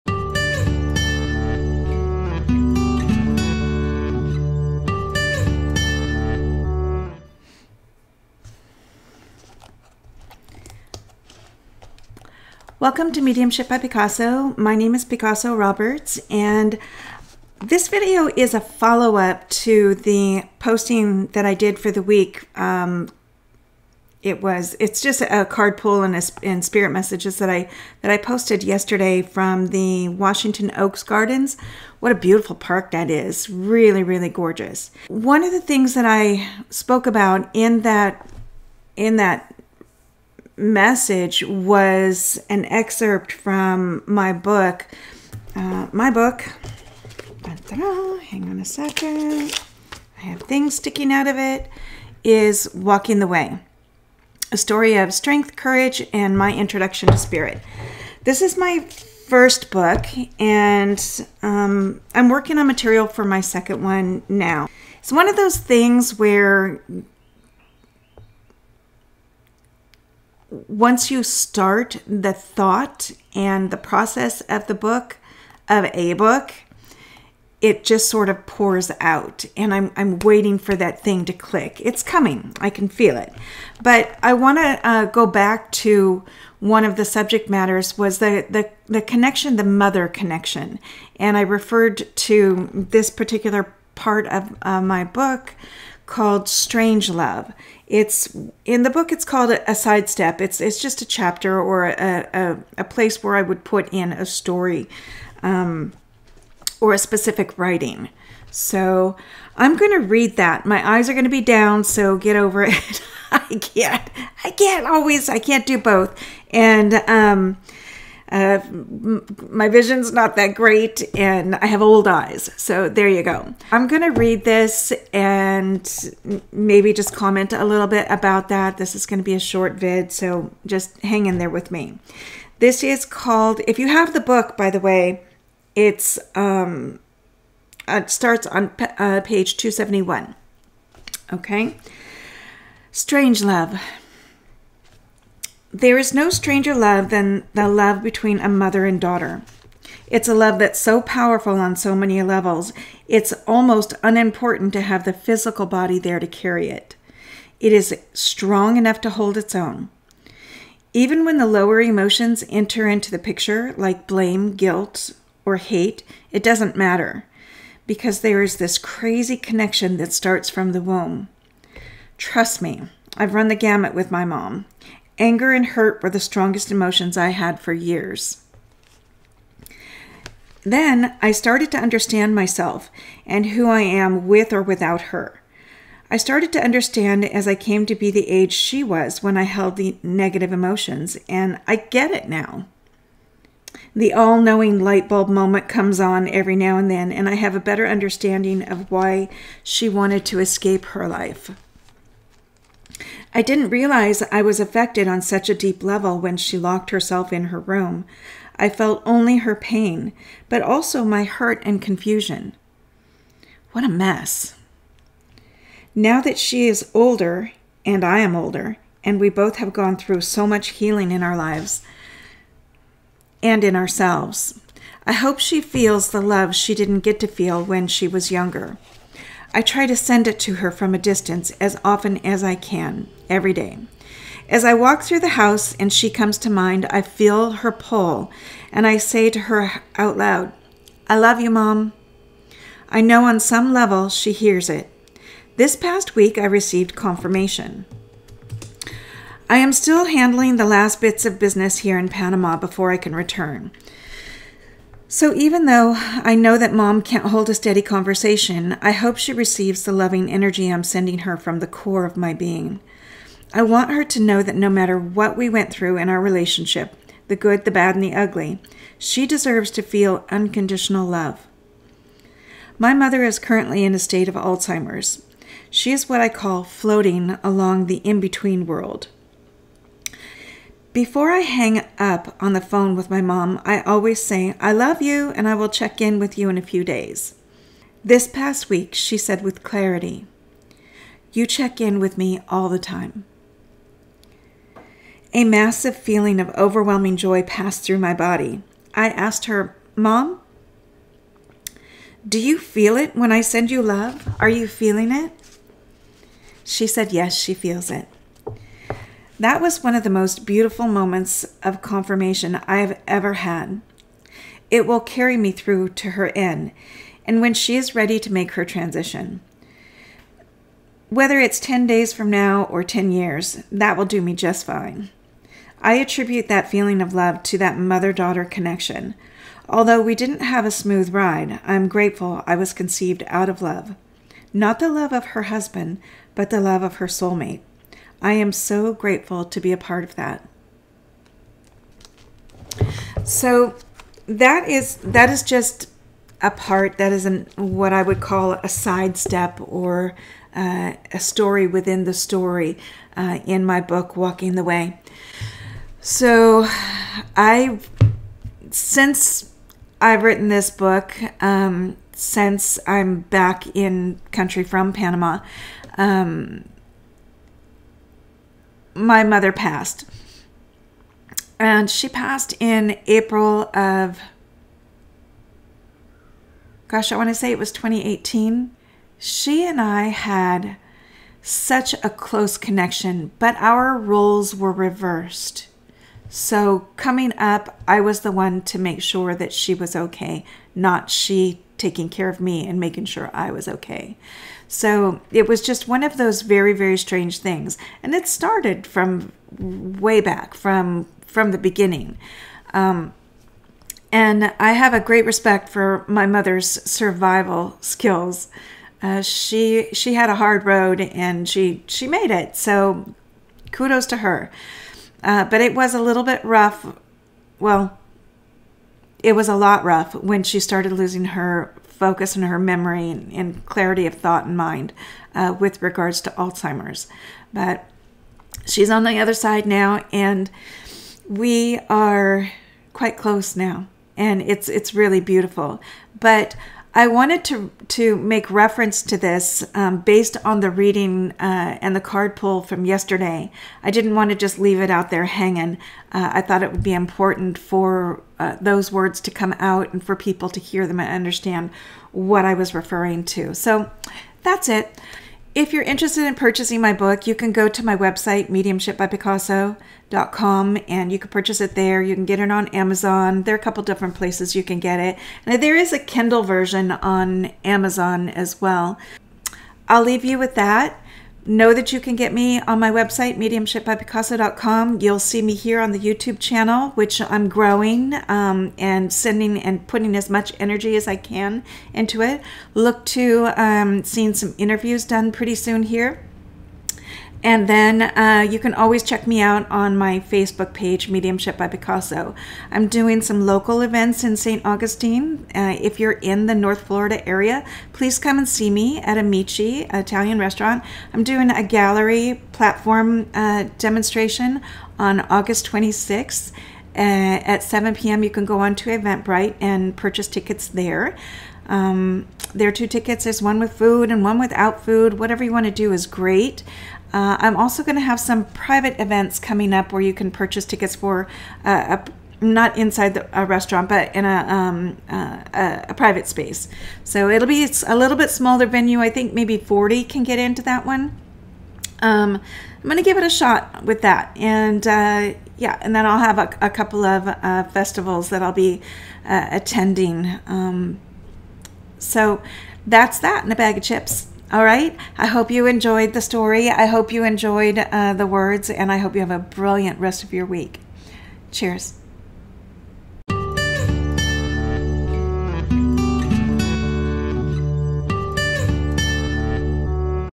Strange Love | Mediumship by Picasso
This is a follow up to the Spirit Messages and card pull from Washington Oaks Gardens 8/15/2022. Referring to the part of my book called "Strange Love". I talk about the mother daughter connection and the strong bond no matter the type of relationship. Its interesting, and I hope you connect and can relate.